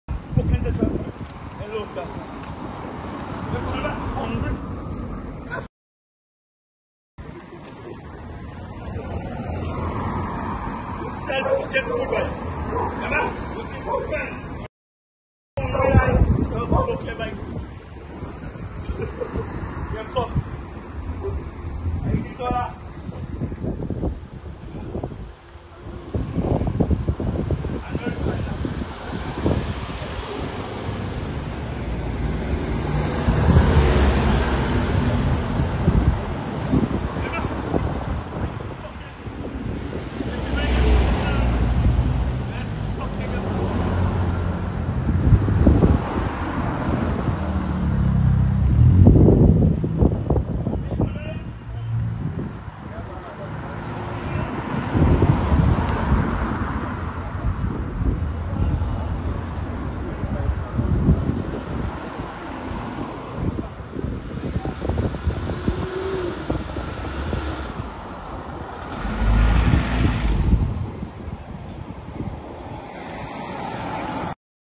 Madman's rant
A “violent” mentally ill man is causing distress at St Michael’s Anglican Primary School in Princes Town.
Parents, residents and vendors are calling on the relevant authority to step in and deal with the cause of the distress